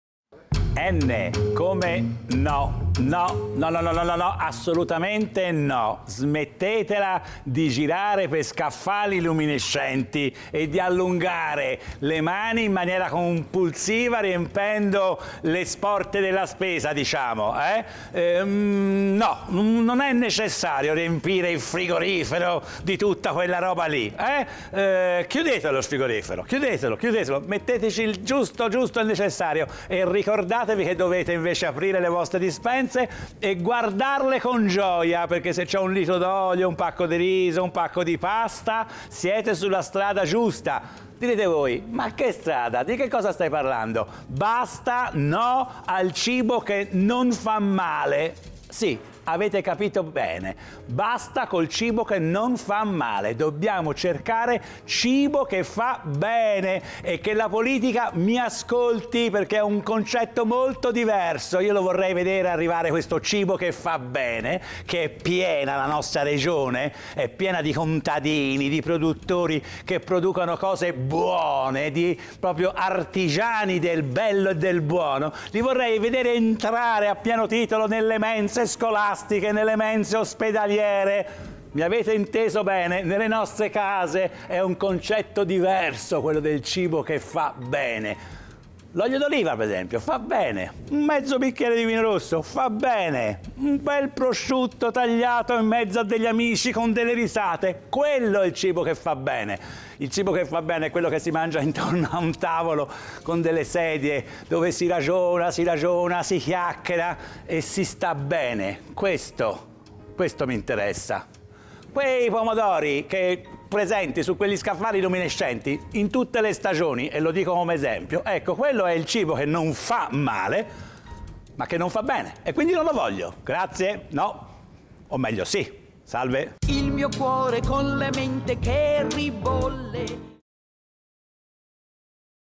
N come No! - Sesta serie
Il vocabolrio in Cucina, materie prime e ricette. No! State lontani da scaffali luminescenti e avvicinatevi alle vostre dispense, basta col cibo che non fa male, sì al cibo che fa bene!